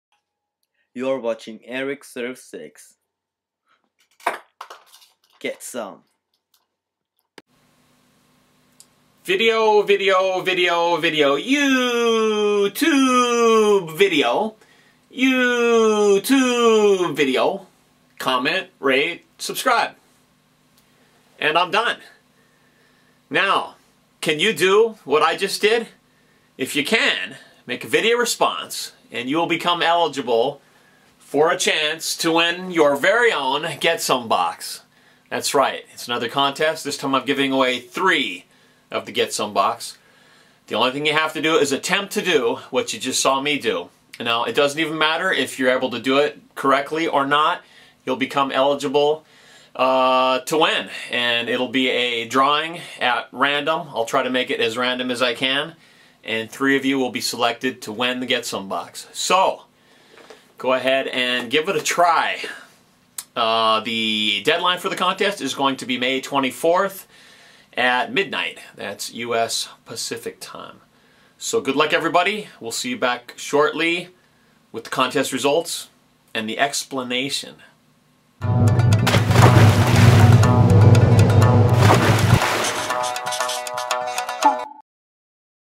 "Do What I Do" Challenge Contest
Can you do what I did?  Contest rules:  Make a video response to this video doing what I did.   Three winners will be selected at random to win the Get Some Box.  Contest deadline for video responses:  May 24, midnight US pacific time.  One video response per channel.

Thanks to apejaffa for the impressive ping pong ball over the shoulder into the dish in the corner trick intro